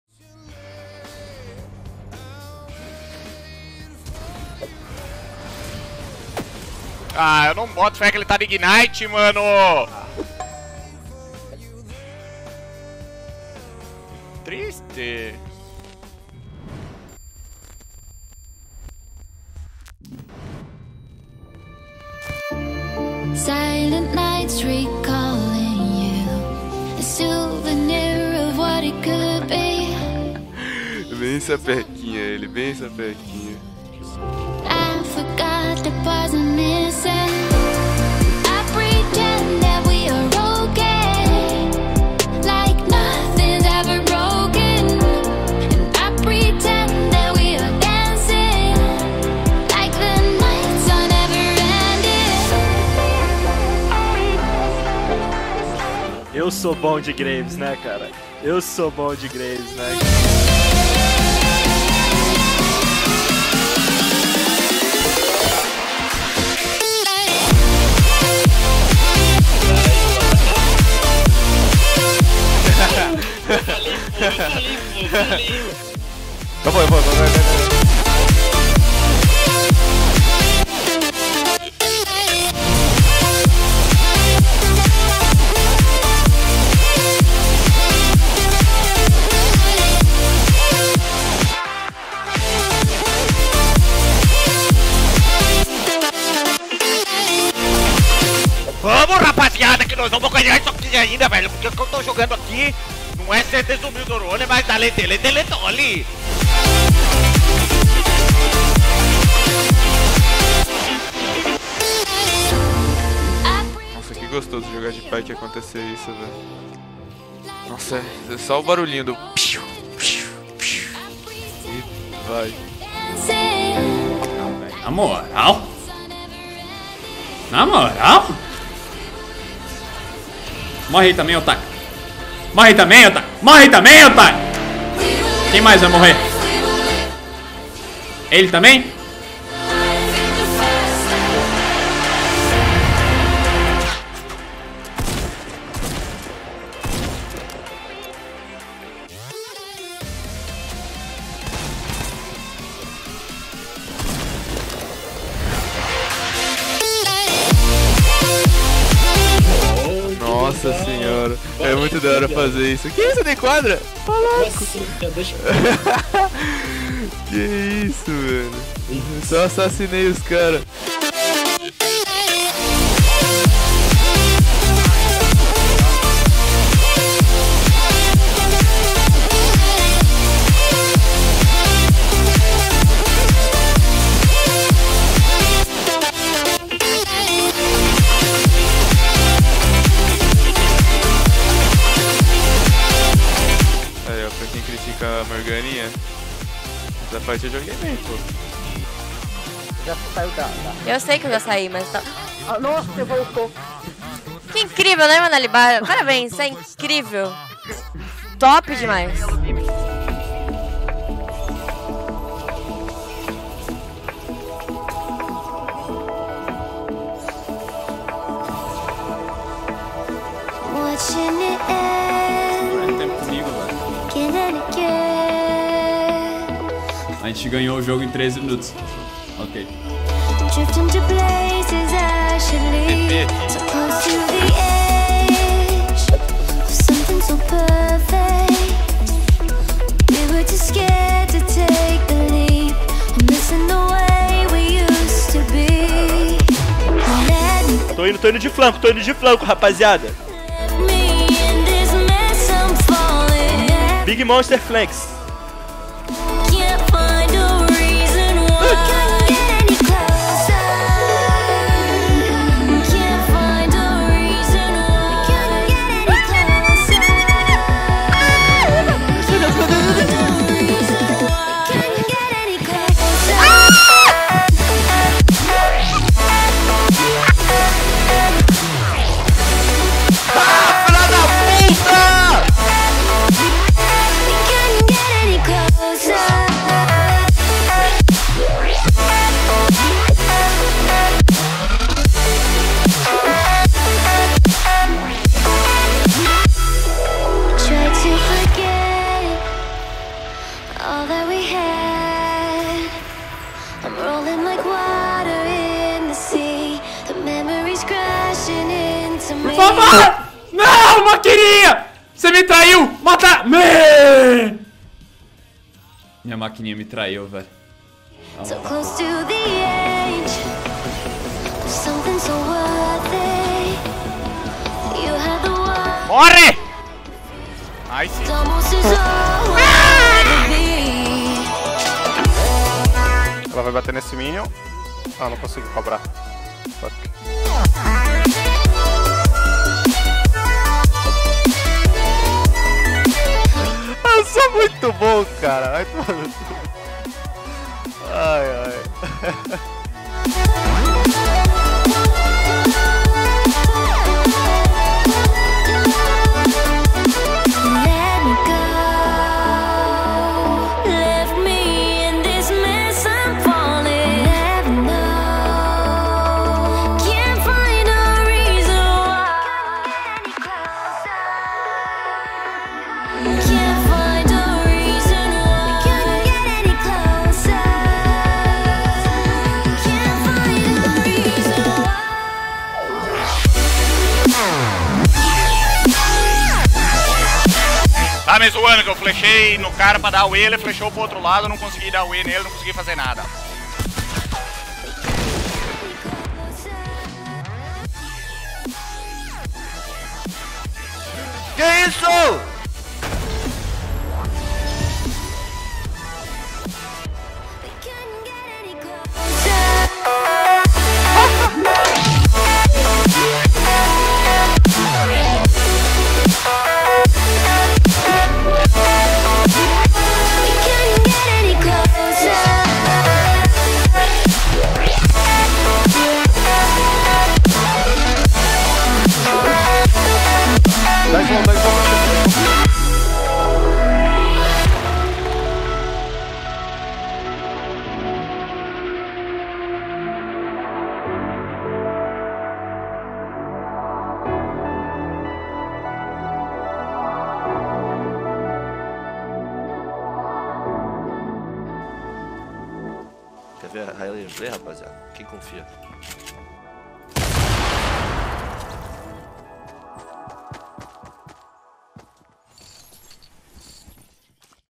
CLIPS #2 COM OOKINA, DANIELS, REDBERT E MAIS
Confiram os melhores lances que aconteceram nas streams esta semana.